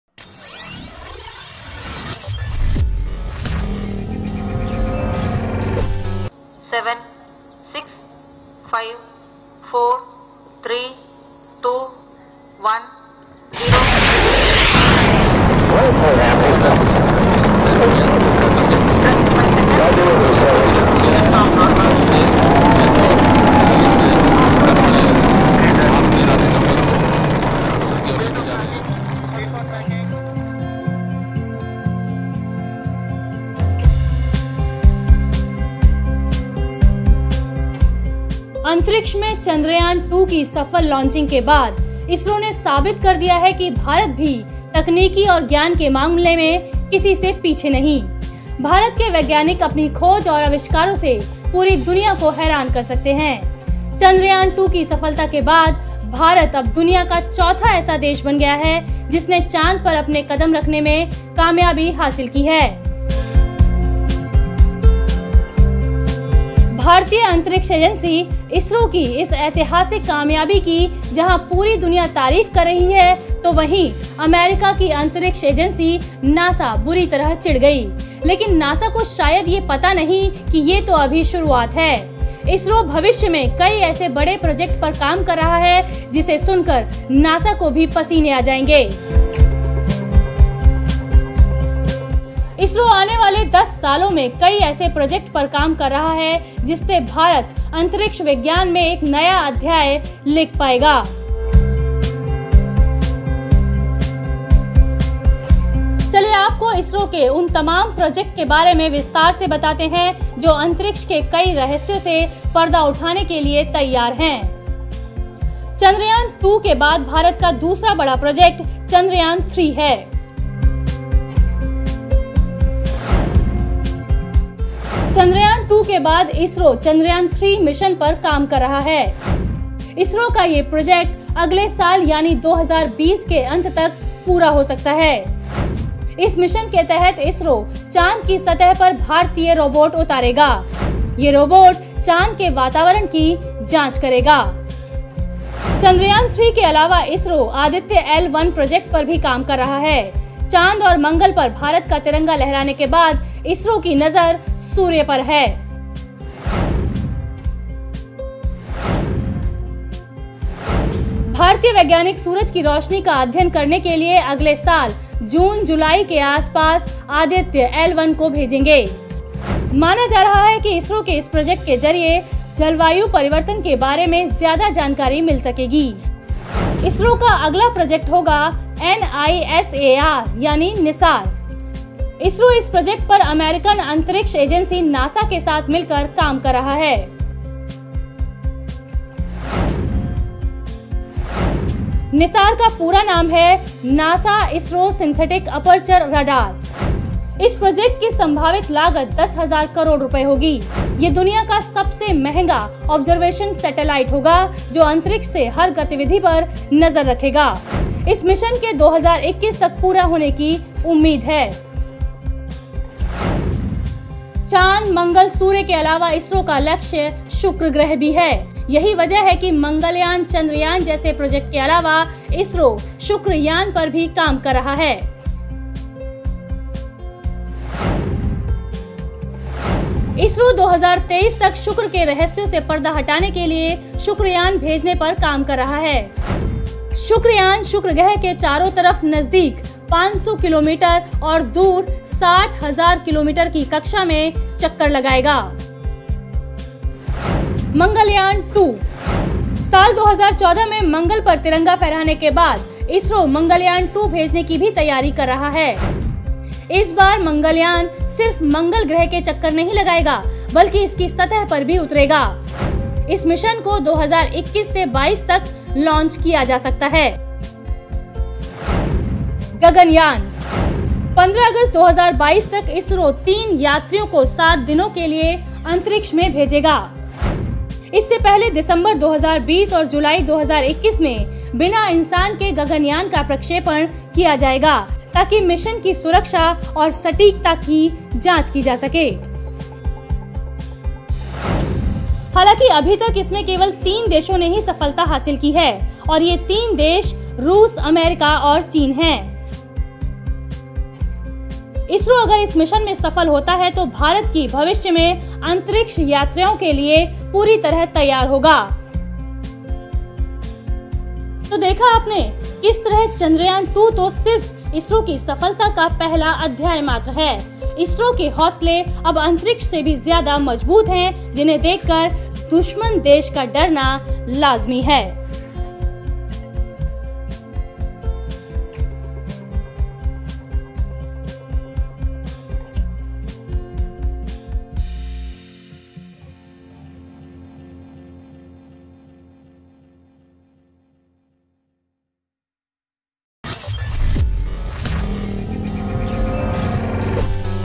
चन्द्रयान-2 ही नहीं, ISRO के भविष्य के ये Project कर देंगे पूरी दुनिया को हैरान
चन्द्रयान टू की शानदार सफलता के बाद पूरी दुनिया भारत की प्रतिभा का लोहा मान रही है…इसरो की ये कामयाबी तो अभी पहला कदम है…भविष्य में वो कई ऐसे प्रोजेक्ट पर काम कर रहा है जिससे नासा भी हैरान हो जाएगा…